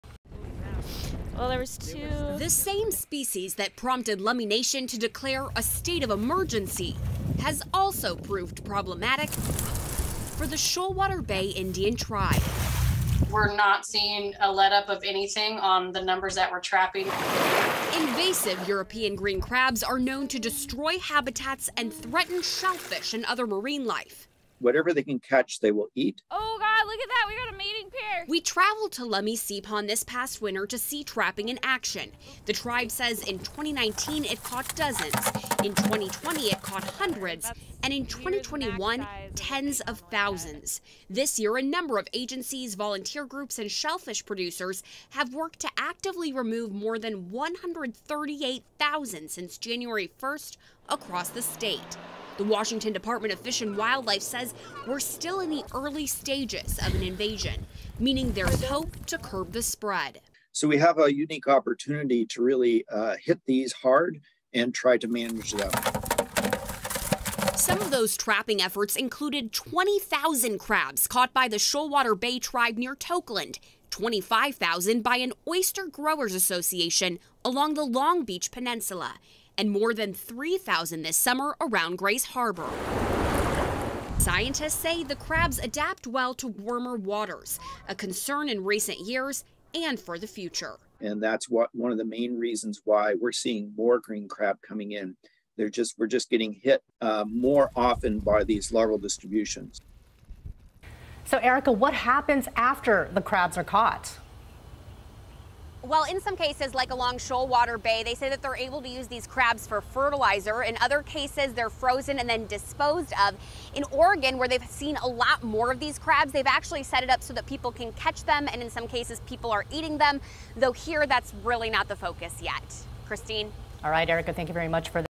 Over 100,000 invasive crabs trapped this year
A number of agencies, tribes and groups across Washington state are working to limit the growth of European green crab populations. Scientists say the invasive species can consume shellfish and other native marine life, and destroy habitats that much of the food web relies on.

The most recent update by the Washington Department of Fish and Wildlife (WDFW) said since the beginning of the year, more than 138,000 crabs have been trapped.

Along with the Washington Department of Natural Resources, U.S. Fish and Wildlife Service and Washington Sea Grant, WDFW said shellfish growers, tribes and other entities have played crucial roles in crab removal.